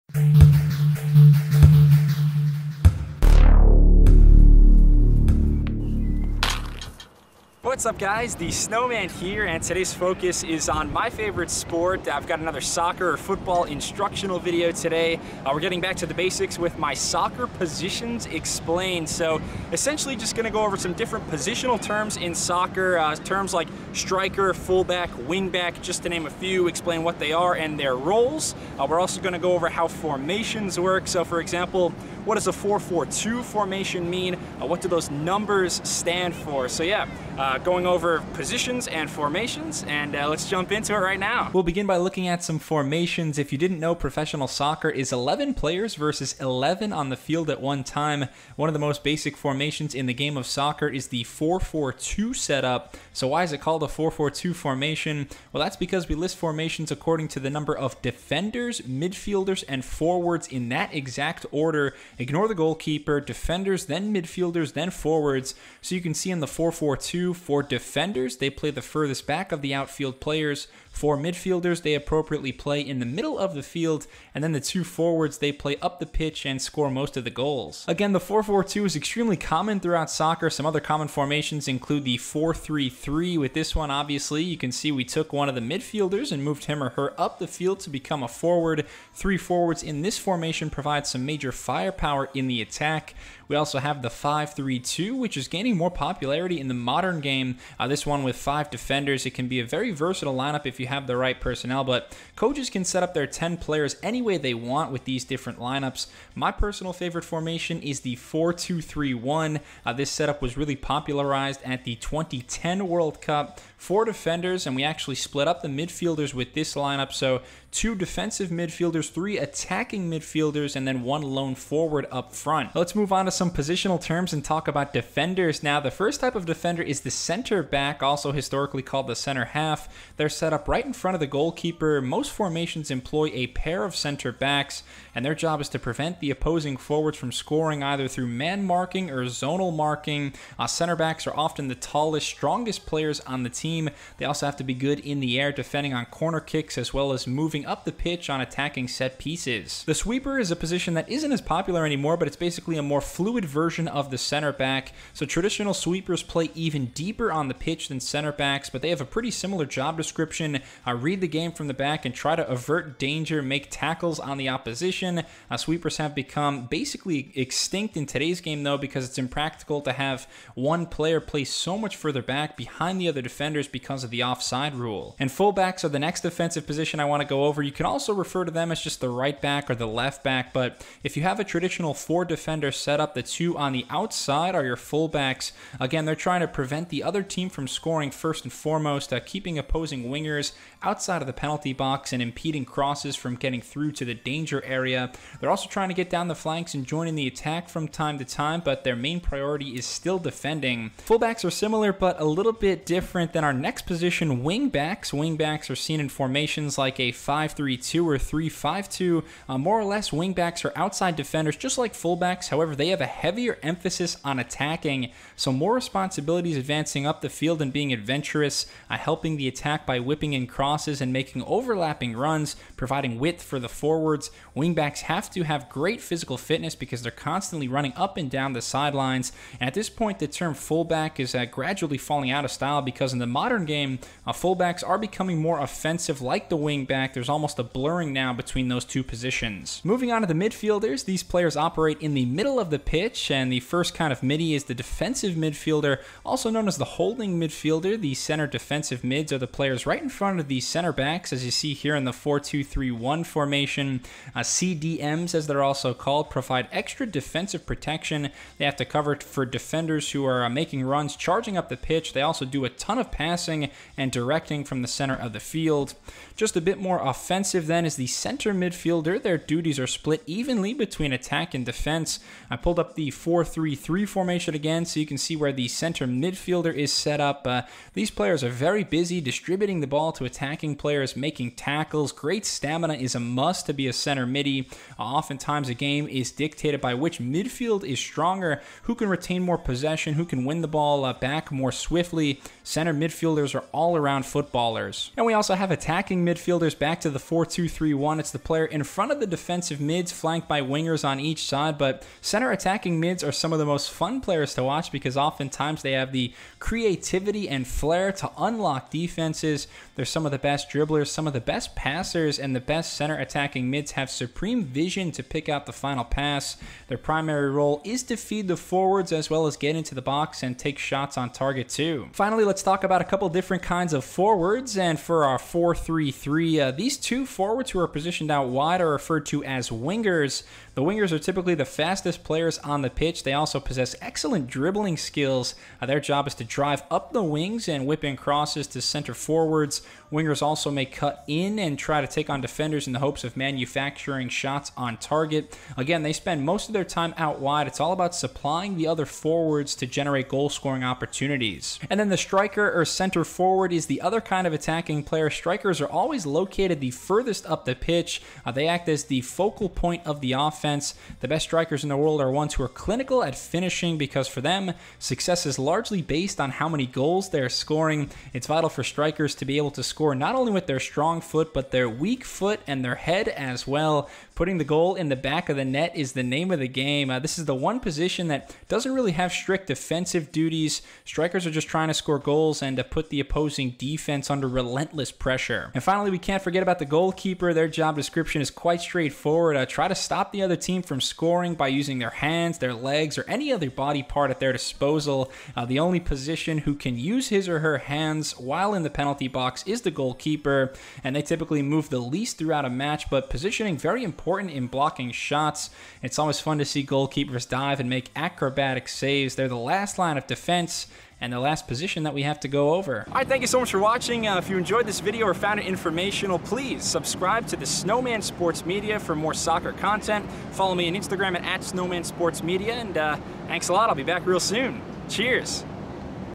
Soccer Positions Explained
Instructional Video breaking down the Different Positions and Formations in the Game of Soccer!